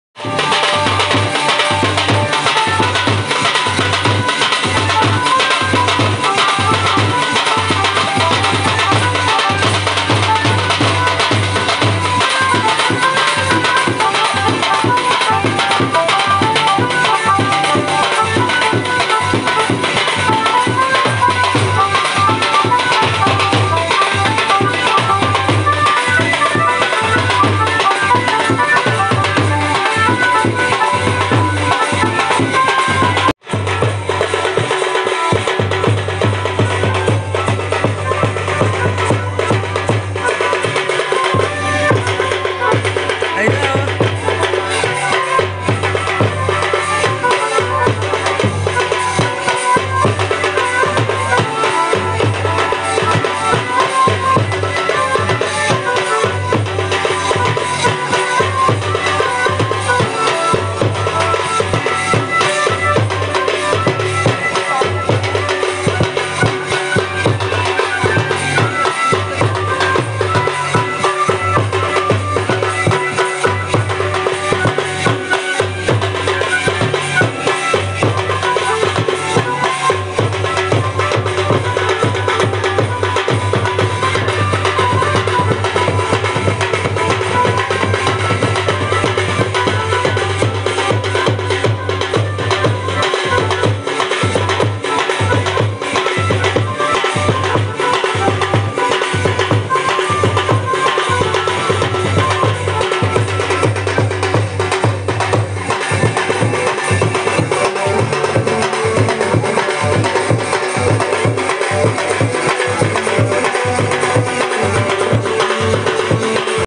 देखो दिन में बहेन के शादी हुई लाकडाउन में Prafulla Mastigrup
गांव की बहेन का देसी शादी देखो कैसे होती है Prafulla Mastigrup

bagheli dance sidhi

                        🤲कलाकार Best🤲